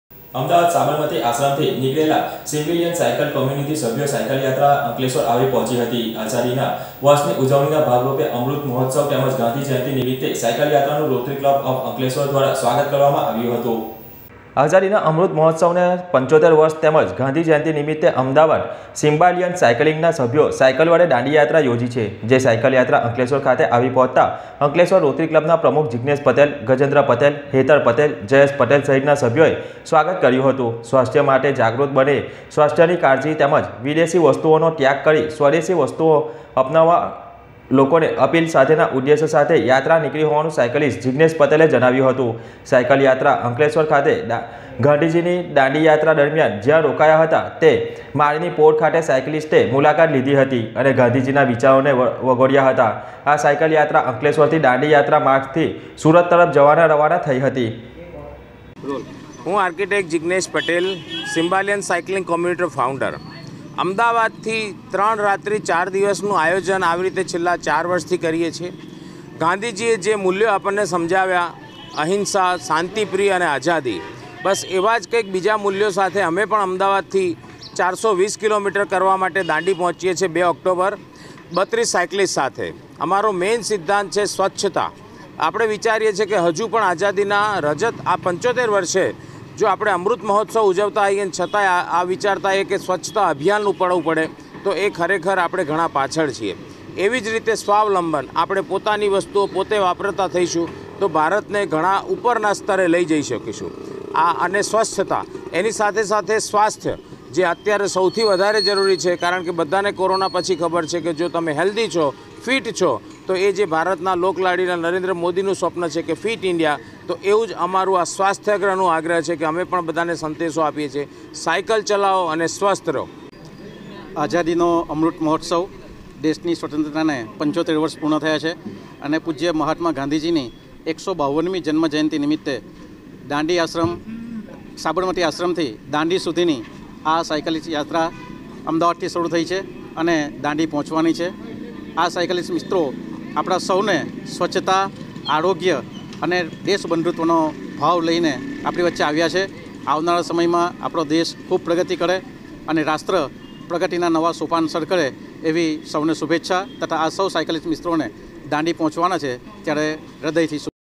માઁ ટીવી 01.10.2021 સિમ્બાલીયન સાઈકલિંગ કોમ્યુનિટી સભ્યો સાઇકલ યાત્રા અંકલેશ્વર આવી પહોંચી
અમદાવાદ સાબરમતી આશ્રમ થી નીકરેલા સિમ્બાલીયન સાઈકલિંગ કોમ્યુનિટી સભ્યો સાઇકલ યાત્રા અંકલેશ્વર આવી પહોંચી

અમૃત મહોત્સવ તેમજ ગાંધી જયંતિ નિમિત્તે સાયકલ યાત્રાનું રોટરી ક્લબ ઓફ અંકલેશ્વર દ્વારા સ્વાગત કરવામાં આવ્યું. : અમદાવાદ થી સાયકલ દ્વારા દાંડી ખાતે નીકળ્યા છે : અંકલેશ્વરમાં દાંડી યાત્રા દરમિયાન ગાંધીજીએ જે જ્યાં વિસામો લીધો હતો તે જ્યોતિ સિનેમા સામે ની માળીનો પોળ ની પણ સાઇક્લિસ્ટ મુલાકાત લીધી